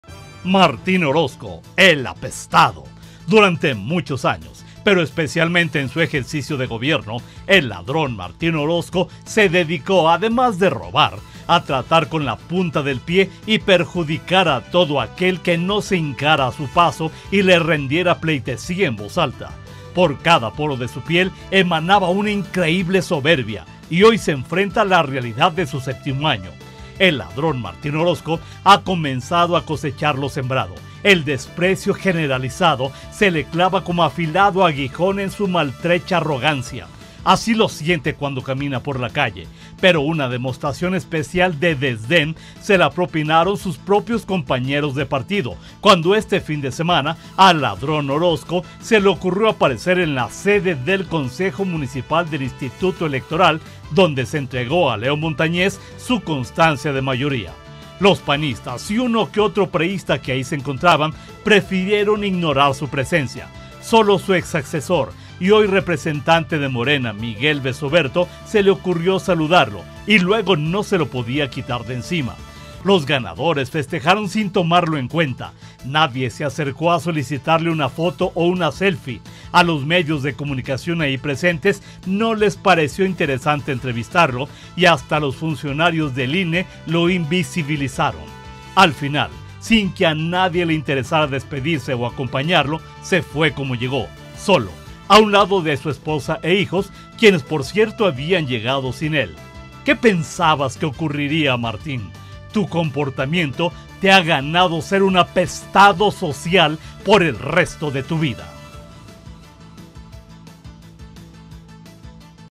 Crónica del Poder/Martín Orozco, el apestado
Martín Orozco, el apestado

Aguascalientes, Ags. 10 de junio 2024.- Durante muchos años, pero especialmente en su ejercicio de gobierno, el ladrón Martín Orozco se dedicó, además de robar, a tratar con la punta del pie y perjudicar a todo aquel que no se hincaba a su paso y le rendía pleitesía en voz alta.
Por cada poro de su piel emanaba una increíble soberbia y hoy se enfrenta a la realidad de su séptimo año.
El ladrón Martín Orozco ha comenzado a cosechar lo sembrado, el desprecio generalizado se le clava como afilado aguijón en su maltrecha arrogancia.
Así lo siente cuando camina por la calle, pero una demostración especial de desdén se la propinaron sus propios compañeros de partido cuando, este fin de semana al ladrón Martín Orozco se le ocurrió aparecer en la sede del Consejo Municipal del IEE donde se entregó a Leo Montañez su constancia de mayoría.
Los panistas y uno que otro priísta que ahí se encontraban prefirieron ignorar su presencia. Sólo a su ex asesor y hoy representante de Morena, Miguel Bess-Oberto se le ocurrió saludarlo y luego no se lo podía quitar de encima.
Los ganadores festejaron sin tomarlo en cuenta, nadie se acercó a solicitarle una foto o una selfie, a los medios de comunicación ahí presentes no les pareció interesante entrevistarlo y hasta los funcionarios del INE lo invisibilizaron.
Al final, sin que a nadie le interesara despedirse o acompañarlo, se fue como llegó: solo, a un lado de su esposa e hijos, quienes por cierto, habían llegado sin él.
¿Qué pensabas que ocurriría, Martín? Tu comportamiento te ha ganado ser un apestado social por el resto de tu vida.